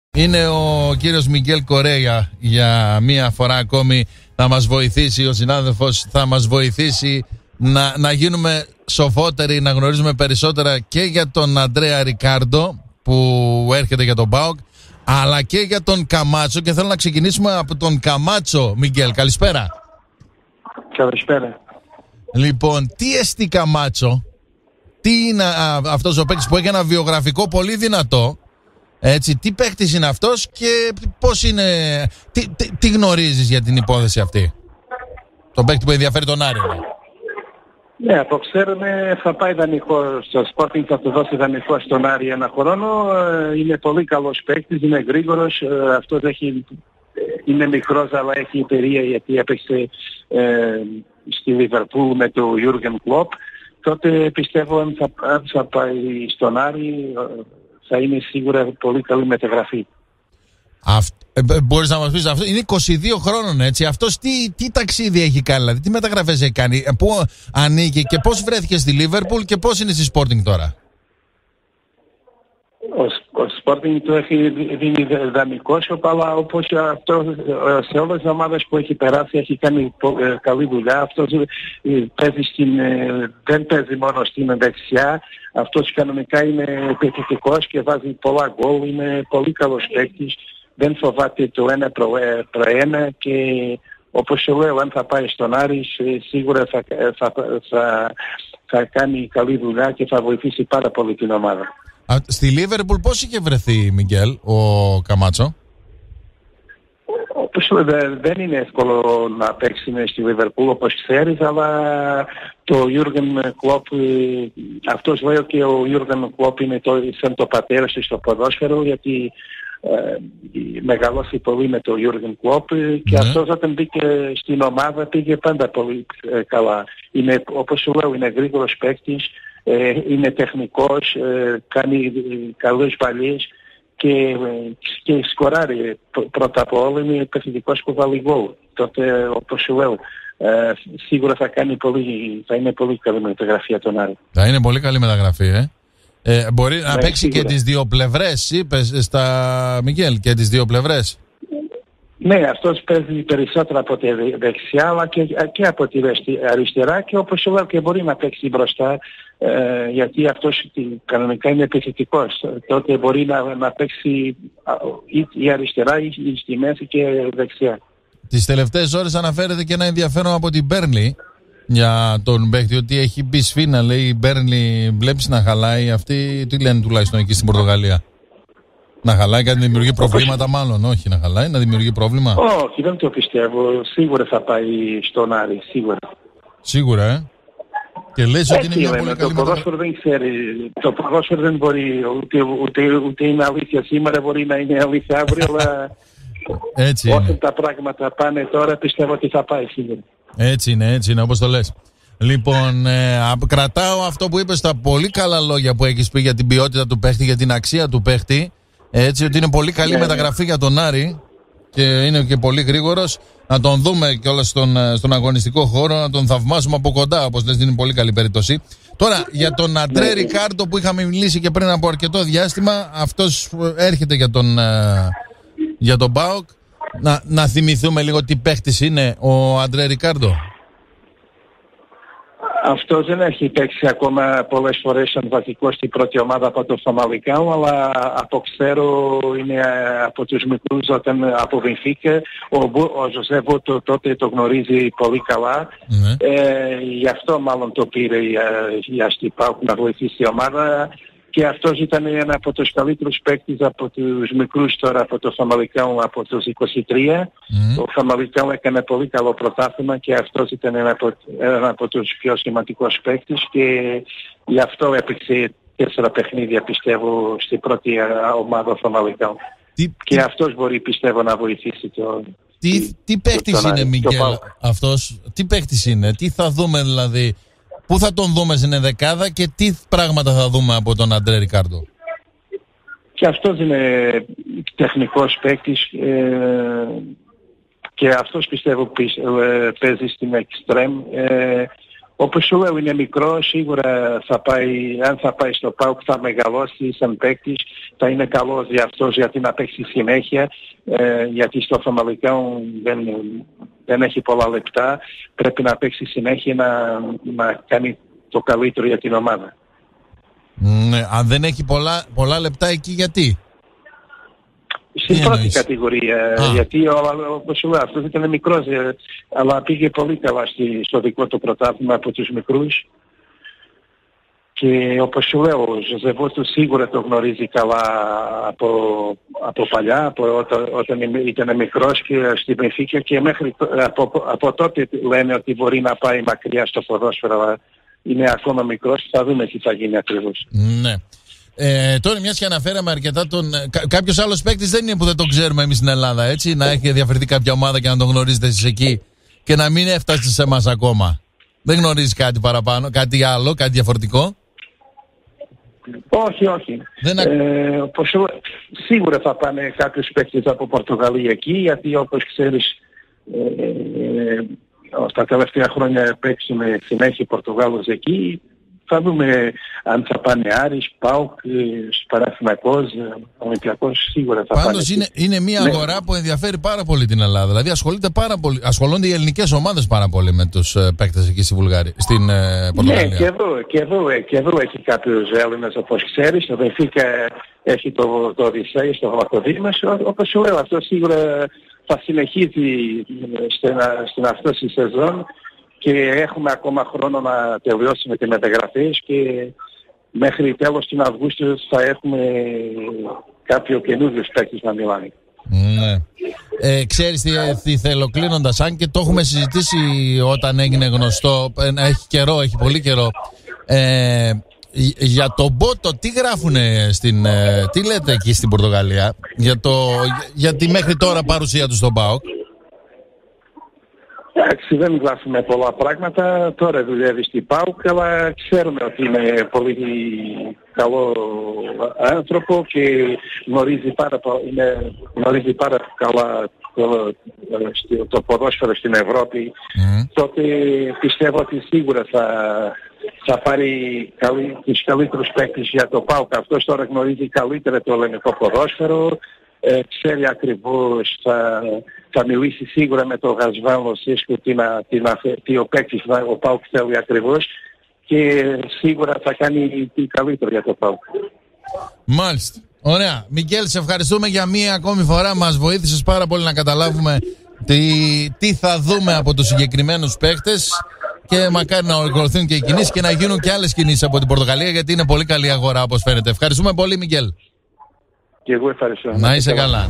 Ο Μιγκέλ Κορέια για Ρικάρντο και Καμάτσο
Για τους Αντρέ Ρικάρντο και Καμάτσο που ο ένας κλείνει στον ΠΑΟΚ και ο άλλος ενδιαφέρει τον Άρη μίλησε στο CONTROL ο Πορτογάλος δημοσιογράφος Μιγκέλ Κορέια. Ακούστε τι είπε σχετικά στον Γιώργο Ζαγόρα στον LIBERO1074 (Δευτέρα-Παρασκευή 12-2 το μεσημέρι).